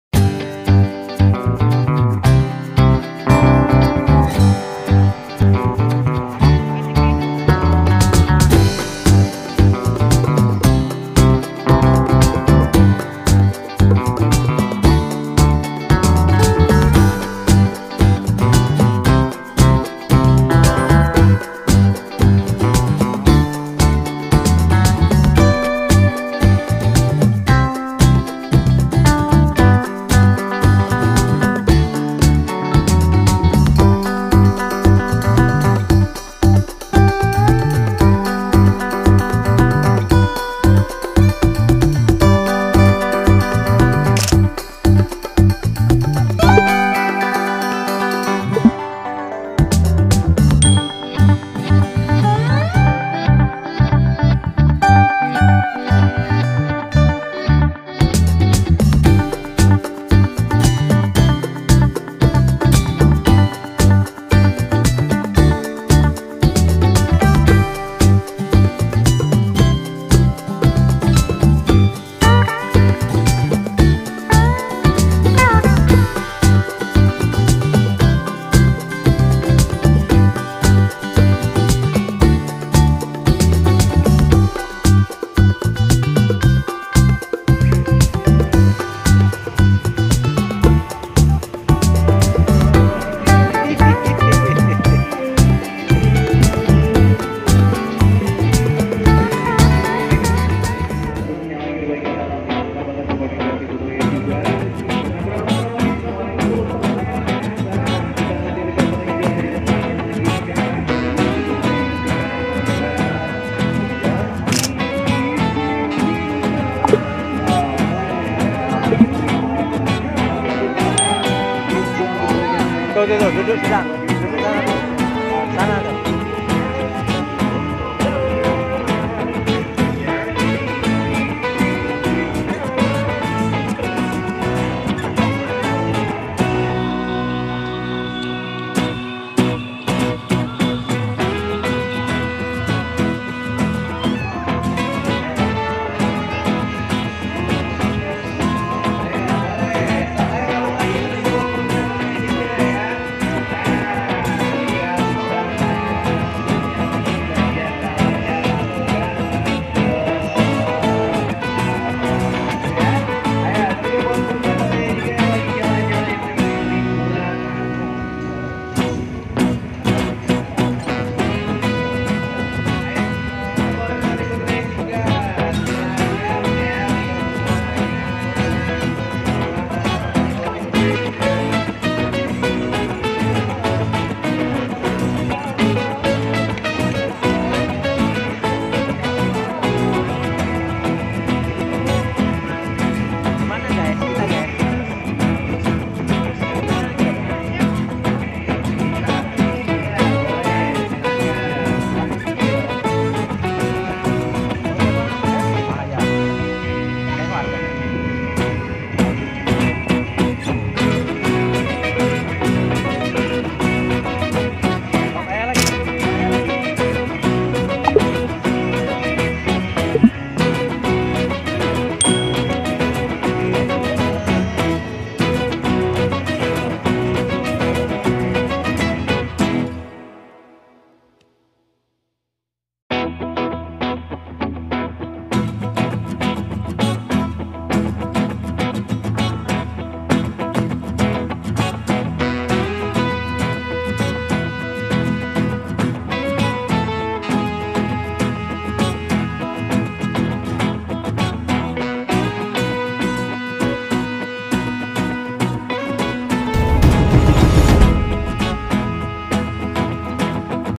Keliling Kota Bandung Pakai Motor || Roadmap Liburan || Alun-alun, Masjid Raya Bandung
Keliling Kota Bandung Pakai Motor || Roadmap Liburan @fitriyani165 @m45fatihEdugame